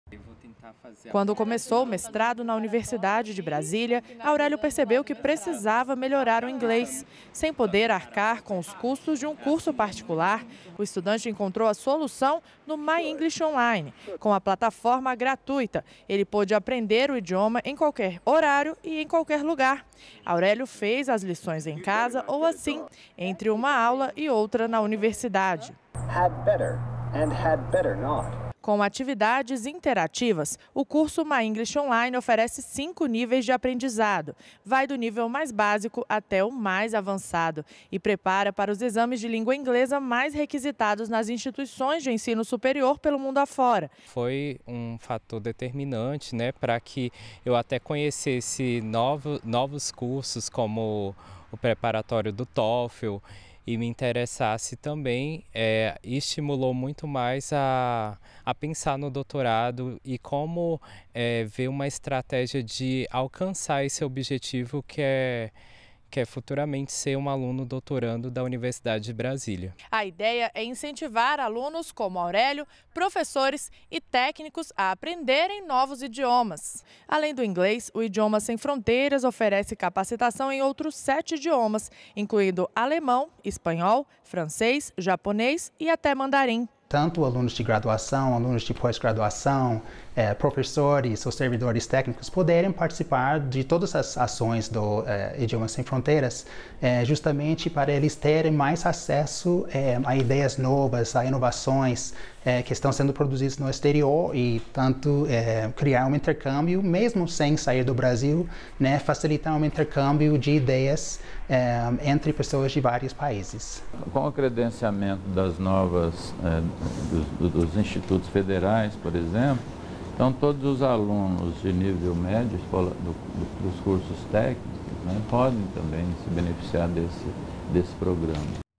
Plataforma do Ministério da Educação oferece curso gratuito para quem quer aprender inglês
REPÓRTER NBR - 03.08.18: O curso está disponível para estudantes de graduação e pós-graduação, servidores de instituições de ensino superior e, agora, também, para alunos matriculados em cursos de escolas técnicas.